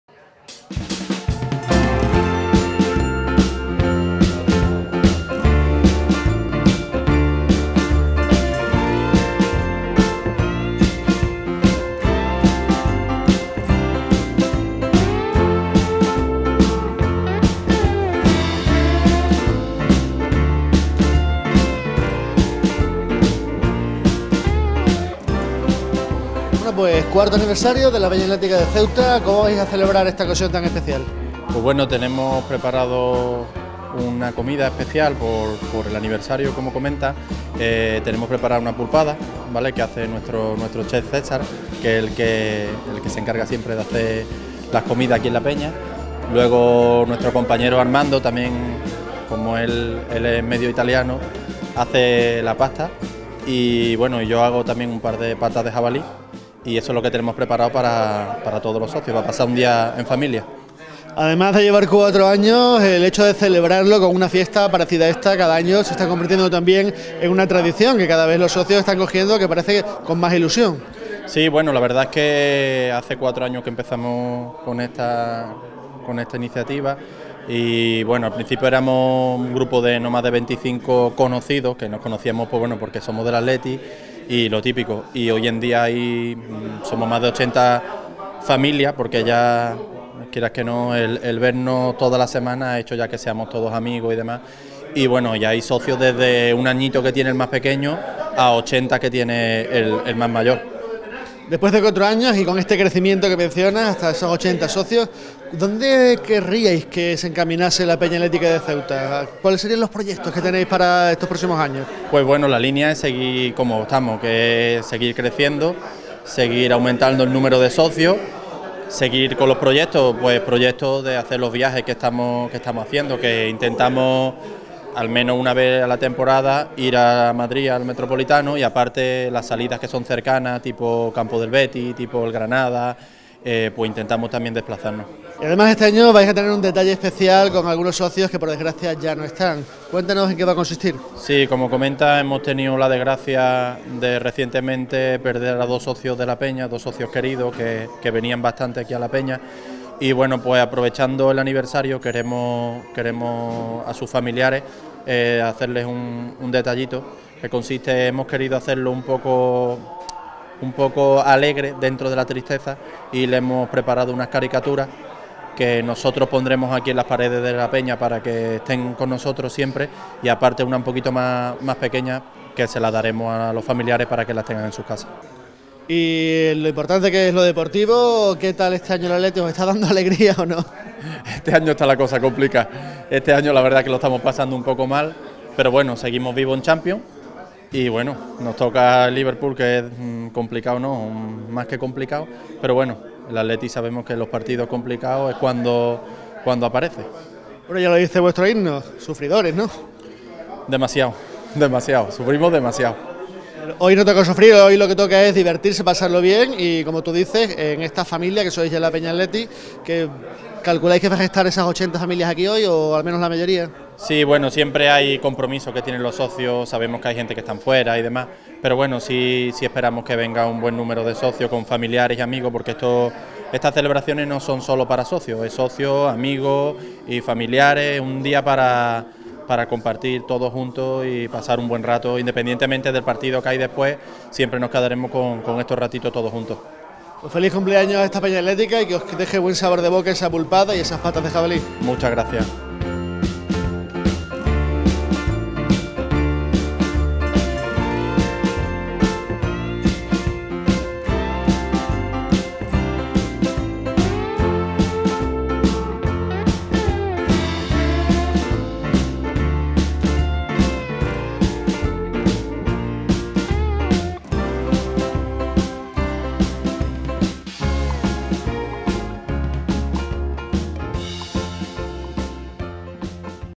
La familia atlética de Ceuta cumple 4 años
La Peña Atlética de Ceuta ha cumplido cuatro años, y lo ha celebrado como es tradición con una degustación para socios y amigos. Este año en Aniversario, han querido recordar con un homenaje a los que causaron baja por fallecimiento, con un detalle lleno de simpatía. Una caricatura que colgarán en la sede, además de regalar copia a las familias.